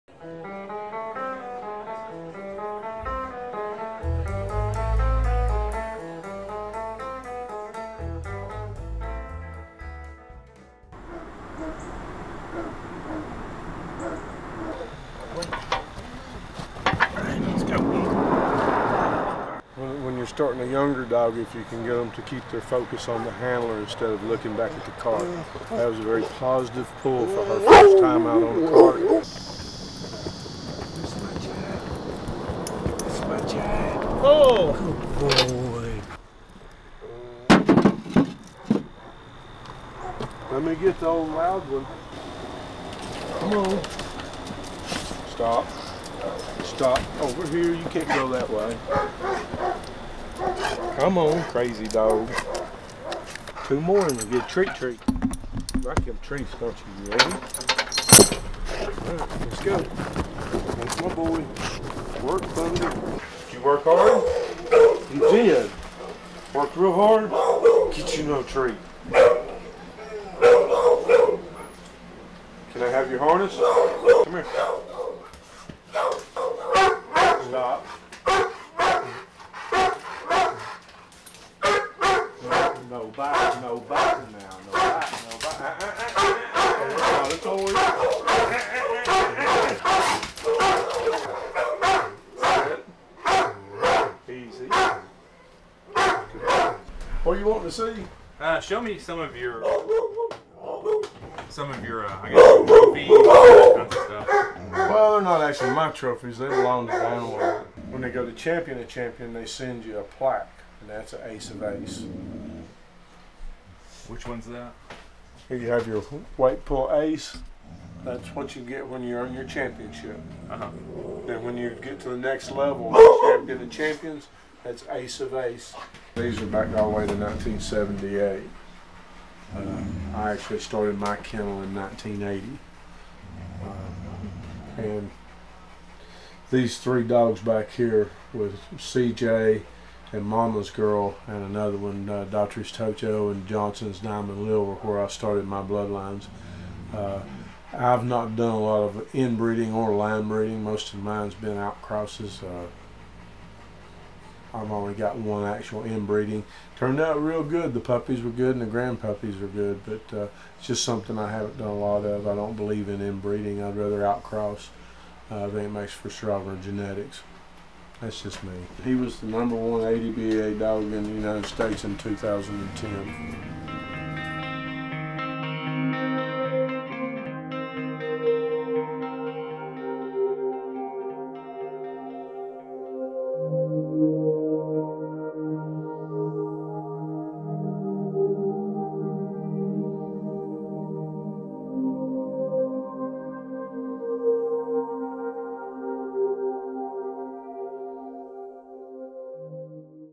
Pit Bull Weight Pull Documentary Teaser #3
Explore the sport of dog weight pulling and the passionate owners that live, eat, sleep, and breathe it.